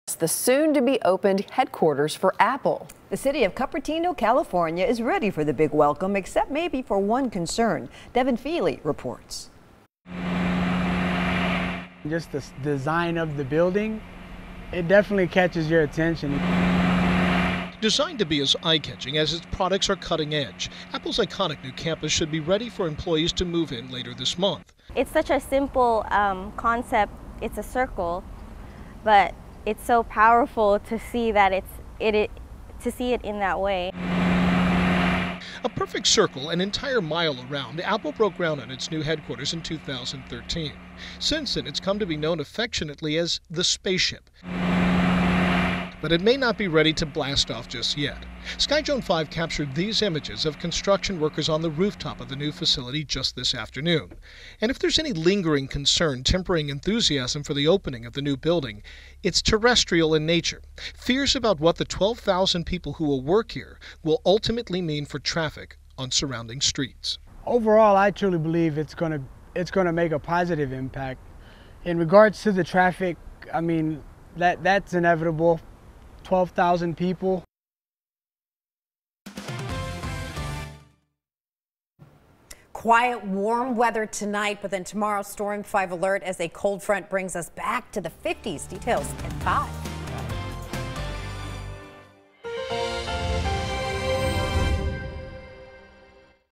Bree's Evening Forecast: Tuesday, April 4, 2017
Bree's evening forecast for Tuesday, April 4, 2017. Scattered showers through the Tuesday overnight hours will pave the way for severe weather Wednesday evening.